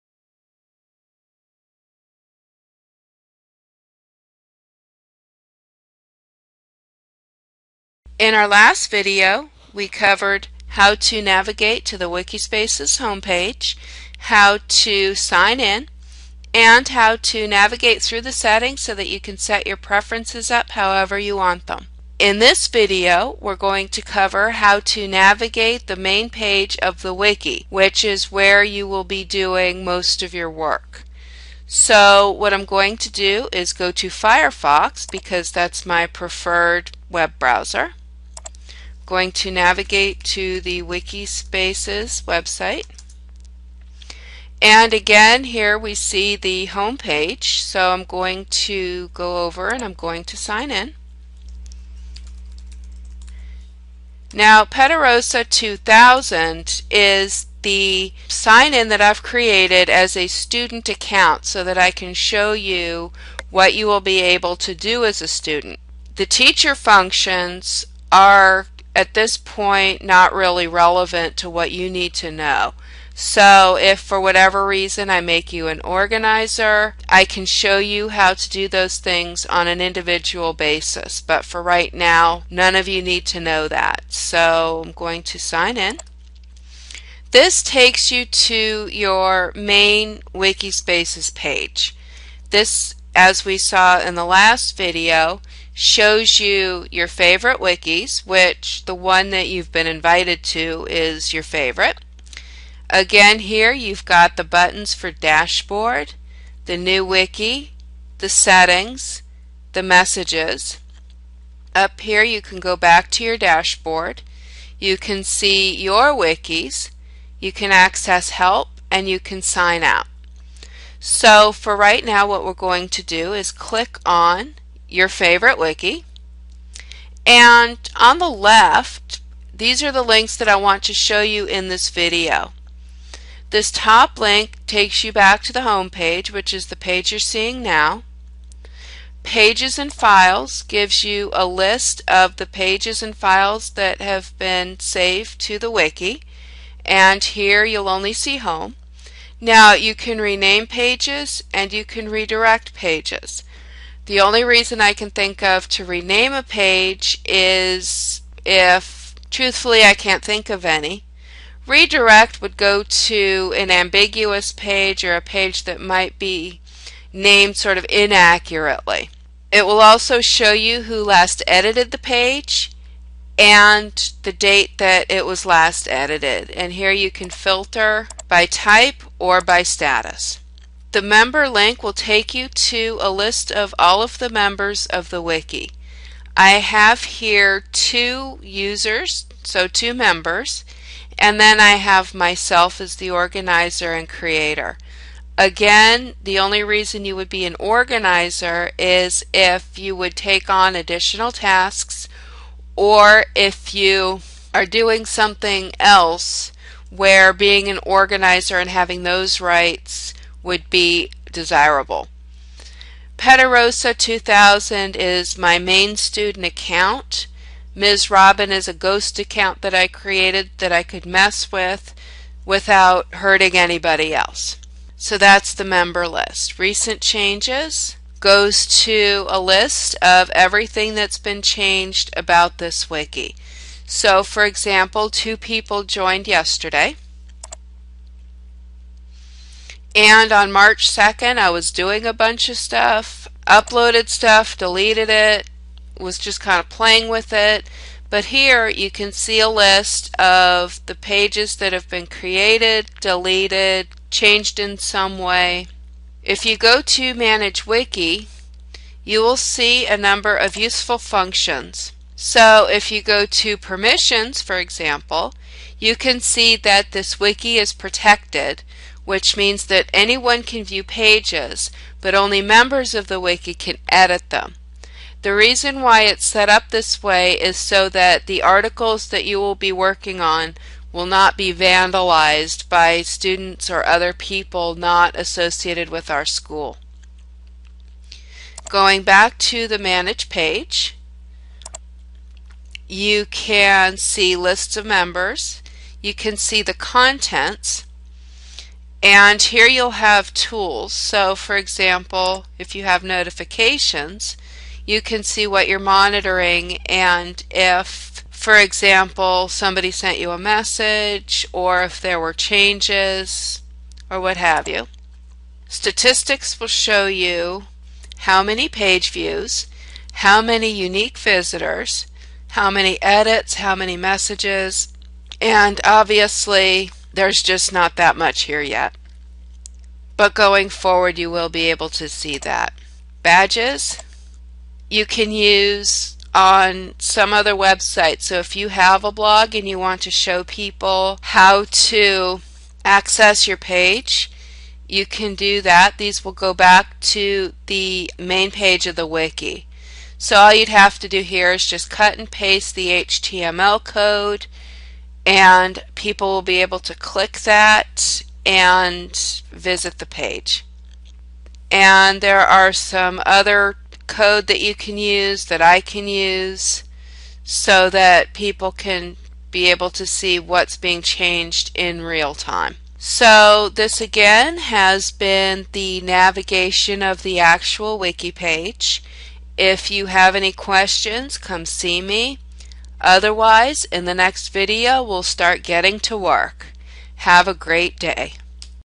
Wikispaces video #2 - Navigating the Wiki's Homepage
This is a short video to demonstrate how to navigate the main page of a wiki.